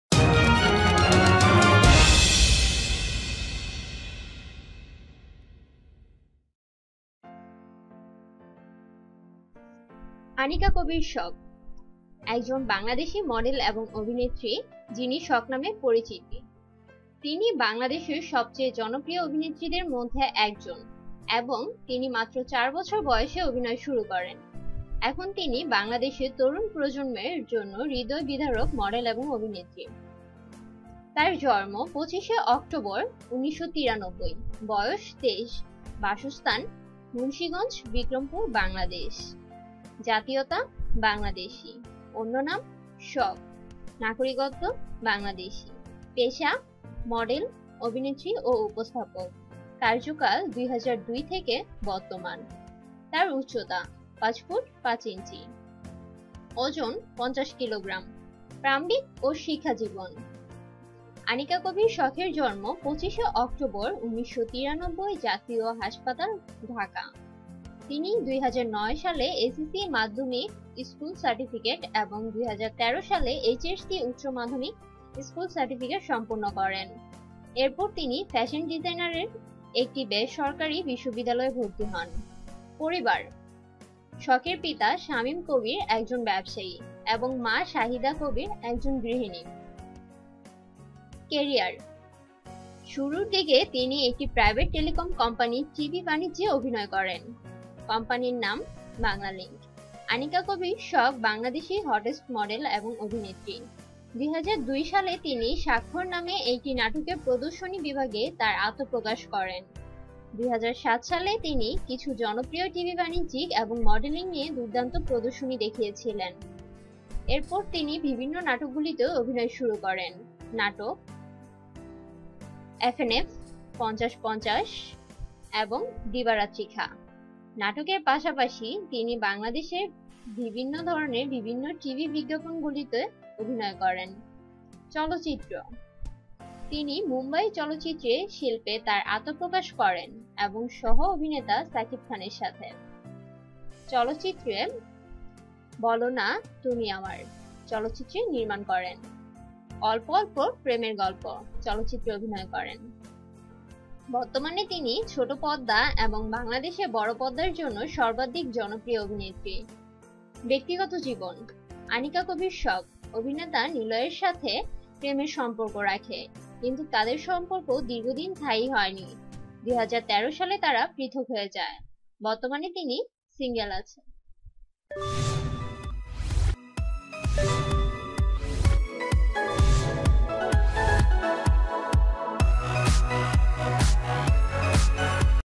অভিনেত্রী আনিকা কবির শখ জীবনী..Model anika kabir shokh Biography personal life lifestyle-2017
Title: অভিনেত্রী আনিকা কবির শখ জীবনী..Model anika kabir shokh Biography, personal life, lifestyle-2017
Description:অভিনেত্রী আনিকা কবির শখ:  একজন বাংলাদেশী মডেল এবং অভিনেত্রী যিনি শখ নামে পরিচিত। তিনি বাংলাদেশের সবচেয়ে জনপ্রিয় অভিনেত্রীদের মধ্যে একজন এবং তিনি মাত্র চার বৎসর বয়সে অভিনয় শুরু করেন। এখন, তিনি বাংলাদেশের তরুণ প্রজন্মের জন্য হৃদয়বিদারক মডেল এবং অভিনেত্রী।
ক্যারিয়ার:
শুরুর দিকে তিনি একটি প্রাইভেট টেলিকম কোম্পানির টিভি বানিজ্যে অভিনয় করেন,
কোম্পানির নাম : বাংলালিংক।
আনিকা কবির শখ বাংলাদেশী হটেস্ট মডেল এবং অভিনেত্রী।
 ২00২ সালে তিনি “শাকখর” নামে একটি নাটকের প্রদর্শনী বিভাগে তার আত্মপ্রকাশ করেন।

২007 সালে, তিনি কিছু জনপ্রিয় টিভি বাণিজ্যিক এবং মডেলিং নিয়ে  দুর্দান্ত প্রদর্শনী দেখিয়েছিলেন।

এরপর তিনি বিভিন্ন নাটকগুলিতে অভিনয় শুরু করেন
নাটক:
যেমন FNF এফএনএফ,
পঞ্চাশ পঞ্চাশ এবং
দিবা রত্রি খাঁ।
নাটকের পাশাপাশি, তিনি বাংলাদেশের বিভিন্ন ধরনের বিভিন্ন টিভি বিজ্ঞাপনগুলিতে অভিনয় করেন।

চলচ্চিত্র:
 তিনি মুম্বাই চলচ্চিত্র শিল্পে তার আত্মপ্রকাশ করেন এবং সহ-অভিনেতা সাকিব খান সাথে
চলচ্চিত্রের
1.
2. বলো না তোমী আমের চলচ্চিত্র নির্মাণ করেন। .
3. Olpo Olpo Premer Golpo চলচ্চিত্রে অভিনয় করেন।

বর্তমানে তিনি ছোট পর্দা এবং বাংলাদেশের বড় পর্দার জন্য সর্বাধিক জনপ্রিয় অভিনেত্রী।

ব্যক্তিগত জীবন:
আনিকা কবির শখ অভিনেতা নিলোয়ের সাথে প্রেমের সম্পর্ক রাখে, কিন্তু তাদের সম্পর্ক দীর্ঘদিন  স্থায়ি হয়নি, ২013 সালে তারা পৃথক হয়ে যায়।
বর্তমান তিনি  Single আছেন..